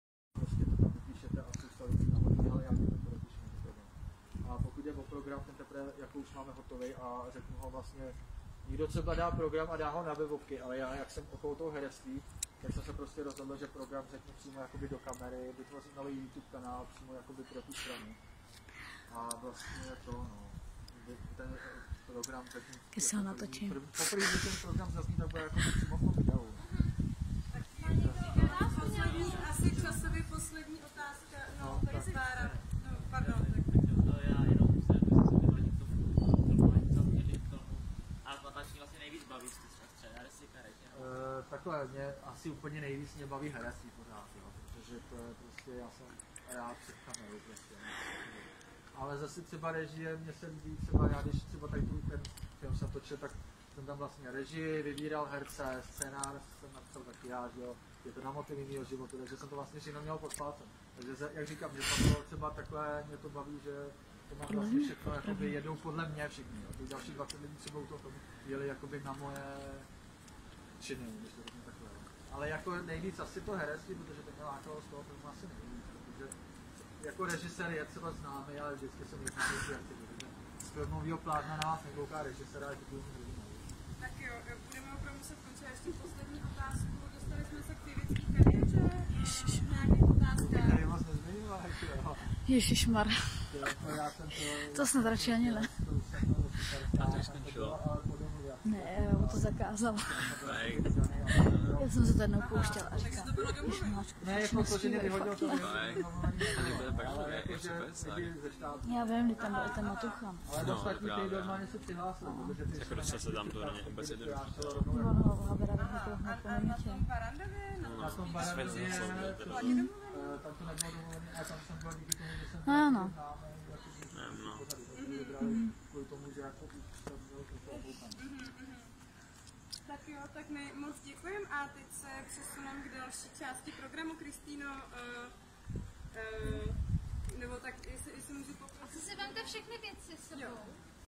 Pavel Dorotka úryvek moderování
filmař Pavel Dorotka,jako host a moderátor na filmovým táboře